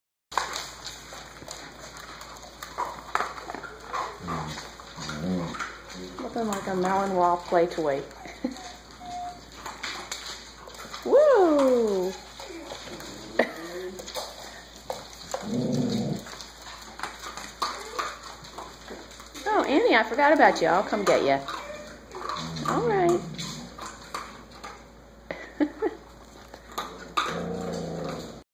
Silken Windhound pups
Silken Windhound pups with their friend and protector, Cleo the Belgian Malinois.